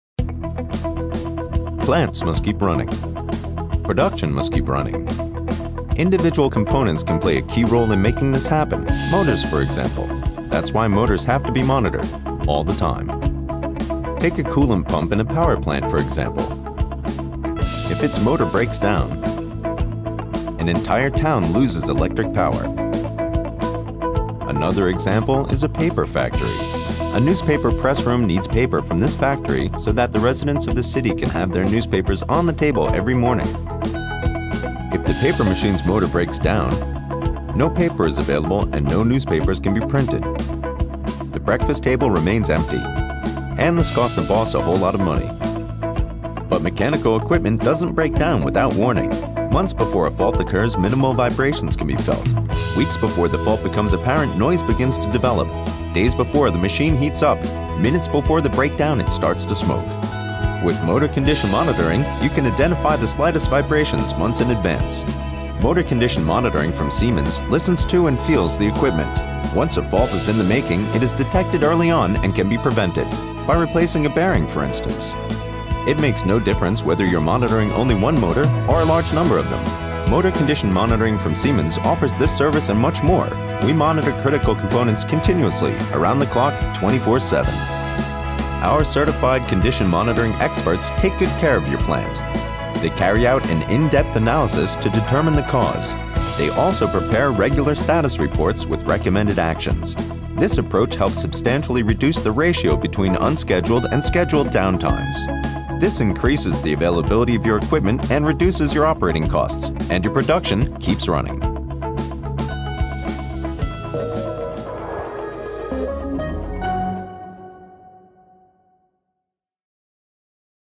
Motor Condition Monitoring
Drive components like motors are typically highly stressed components that are exposed to varying levels of constant wear and tear, depending on the service life and load. Continuous monitoring makes it possible to recognize changes in their operating conditions early on and take appropriate action to prevent costly plant shutdowns and production losses. Within the scope of a service contract, Siemens Industry Services offers a condition monitoring portfolio with both standardized and customized solutions for plants of any size.